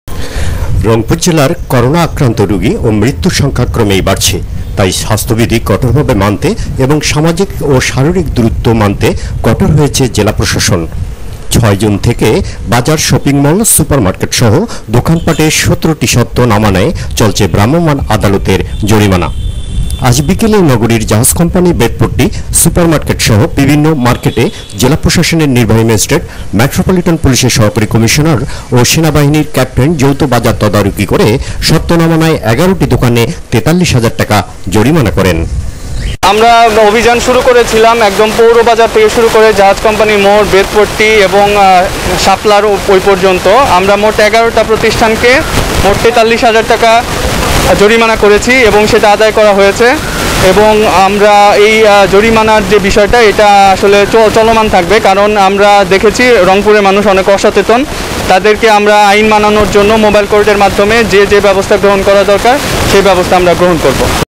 রংপুরে জেলা প্রশাসনের ভ্রাম্যমান আদালত অব্যাহত
রংপুর জেলায় করোনা আক্রান্ত রোগী ও মৃত্যুর সংখ্যা ক্রমেই বাড়ছে।  তাই স্বাস্থ্যবিধি কঠোর ভাবে মানতে এবং সামাজিক ও শারীরিক দুরত্ব মানতে কঠোর হয়েছে জেলা প্রশাসন। ৬ জুন থেকে বাজার,শপিং মল,সুপার মার্কেটসহ দোকান পাটে ১৭ টি শর্ত না মানায় চলছে ভ্রাম্যমান আদালতে জরিমানা।
আজ বিকেলে নগরীর জাহাজ কোম্পানী,বেতপট্টি,সুপার মার্কেটসহ বিভিন্ন মার্কেটে জেলা প্রশাসনের নির্বাহী ম্যাজিষ্ট্রেট,মেট্রোপলিটন পুলিশের সহকারী কমিশনার ও সেনাবাহিনীর ক্যাপ্টেন যৌথ বাজার তদারকি করে শর্ত না মানায় ১১টি দোকানে ৪৩ হাজার টাকা জরিমানা করেন।